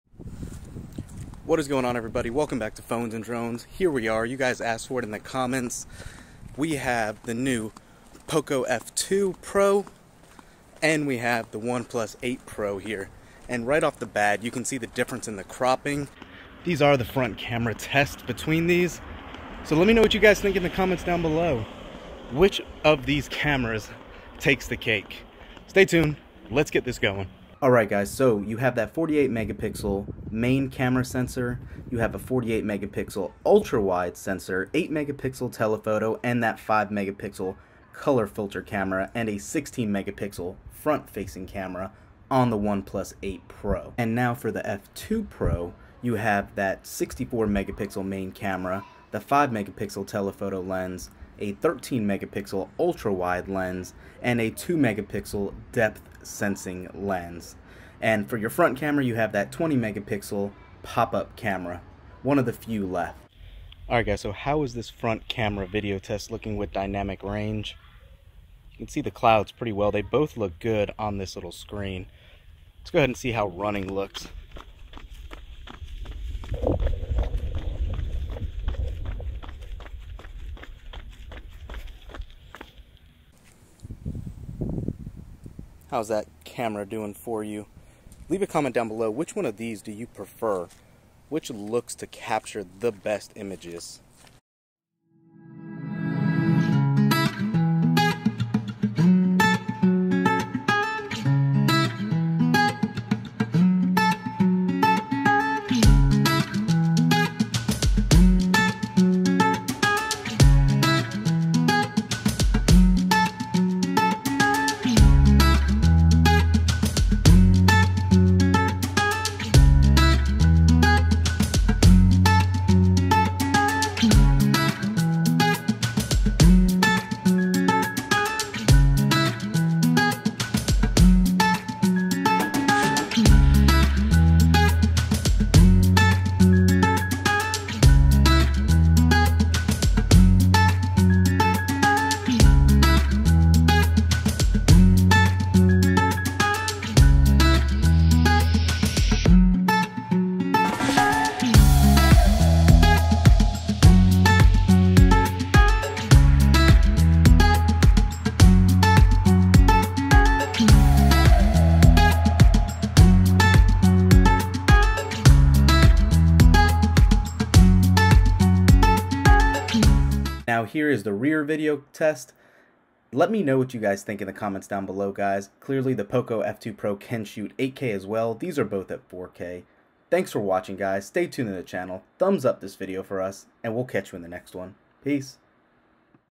POCO F2 Pro Vs OnePlus 8 Pro | Camera Masters? | Camera Test Comparison
These are 2 high end devices in complete different price tiers. Can the brand new Poco F2 Pro really keep up with or BEAT the OnePlus 8 Pro?
Use this code to get exclusive gift - oneplus8yp8at